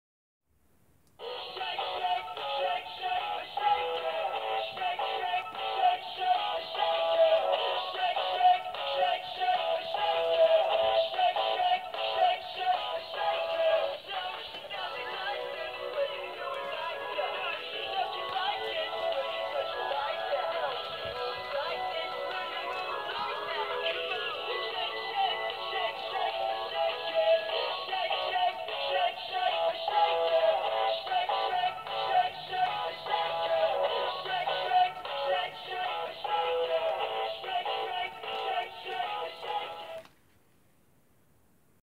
2021 Morrisons Singing Cat
Name: Singing Cat

Sold at: Morrinsons

Year: 2021

Song: I'm not sure what it's called.

About: Cool item new for this year, I can't tell which company produce the items for them but there is quite a range of good and bad quality this year.